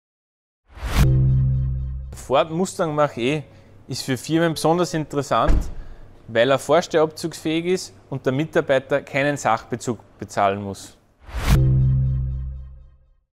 Ford Mustang Mach - E auch als Dienstwagen tauglich ?? | Ford Danner
Das der Ford Mustang Mach - E seine Vorzüge bei Performance & Ausdauer hat, ist ja schon bekannt - aber auch Dienstwagen-Nutzer genießen einige Vorteile 👈 keine NoVA, keine motorbezogene Versicherungssteuer & 0% Sachbezug 👏